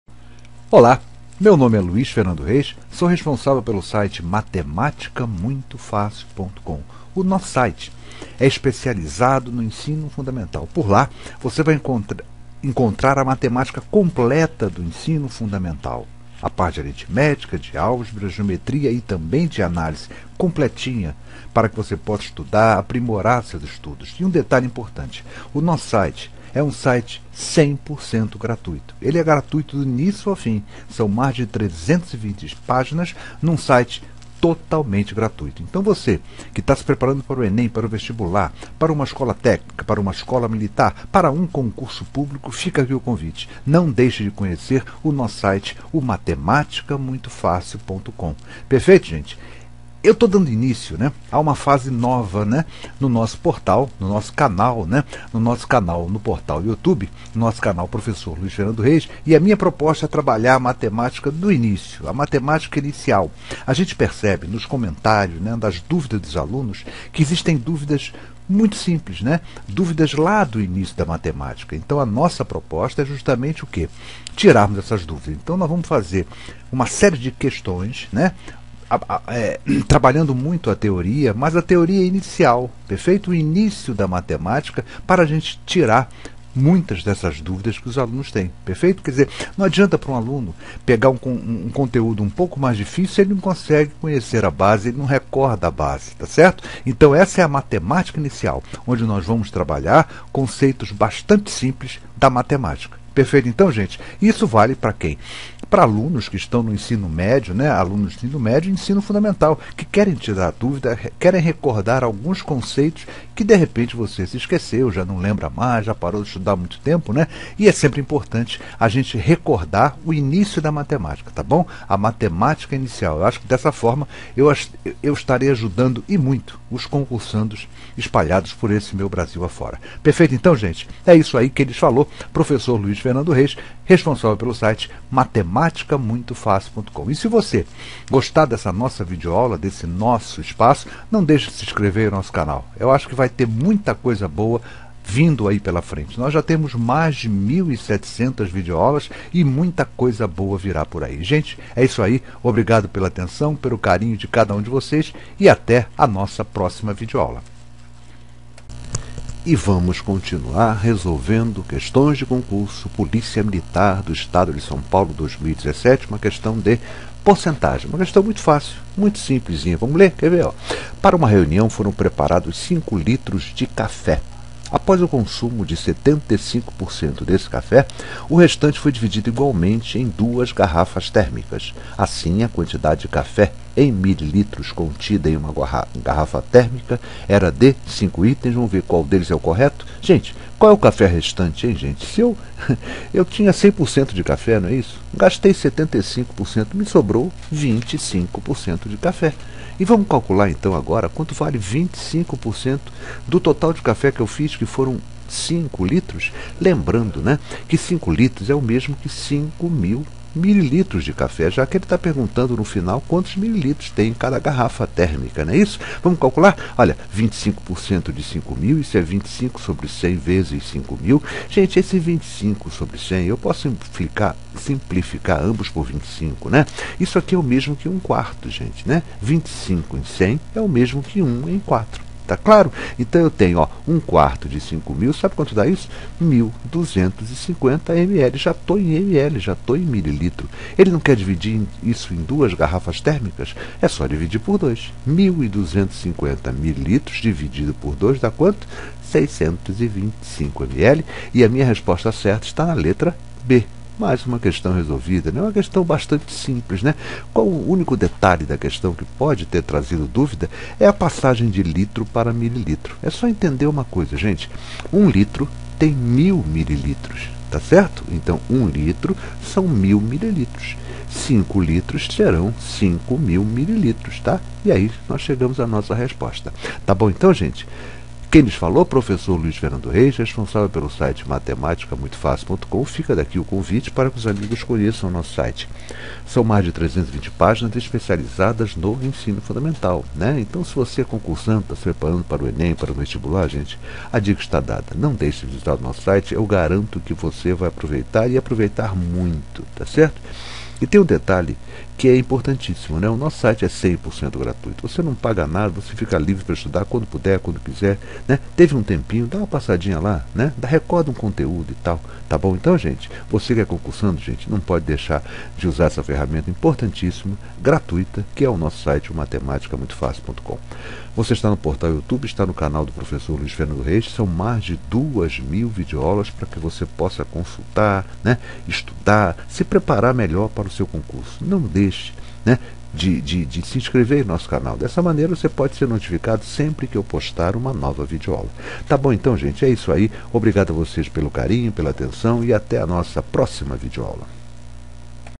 # 2017 15 Polícia Militar de São Paulo 2017 Porcentagem Questão 15
# 2017 15 Polícia Militar de São Paulo 2017 Porcentagem Questão 15 - Videoaula Explicada pelo professor Luiz Fernando Reis do Vídeo educacional e livre para qualquer idade.Videoaula Explicada pelo professor Luiz Fernando Reis.